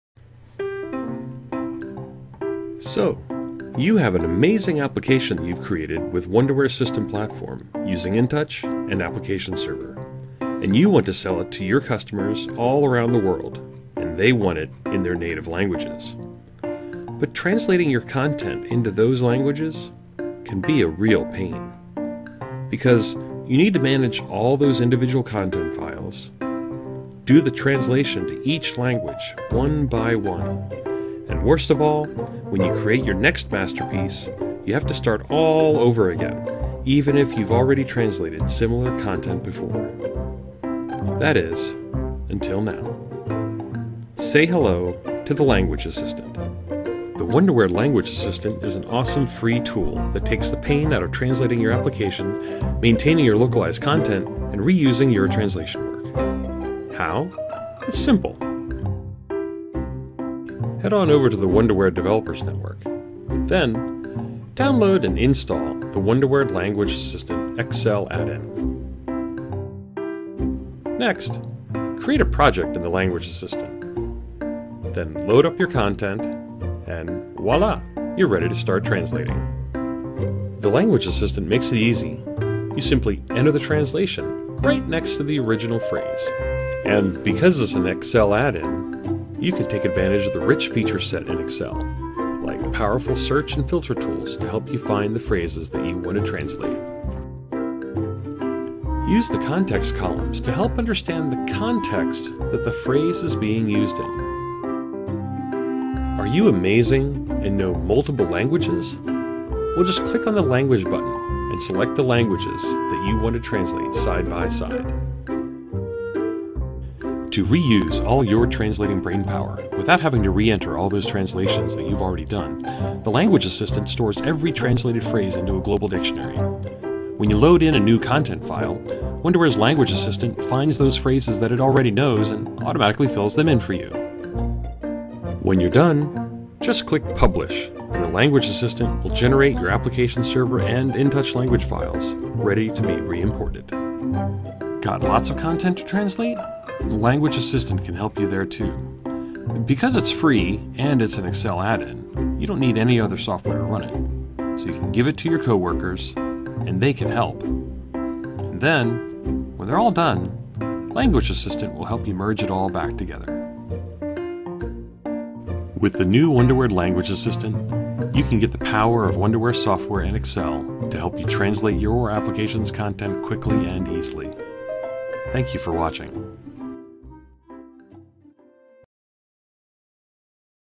Wonderware Language Assistant
An introduction to the new Wonderware Language Assistant, an Excel Add-In designed to assist in the translation of application content from Wonderware InTouch and the Wonderware Application Server.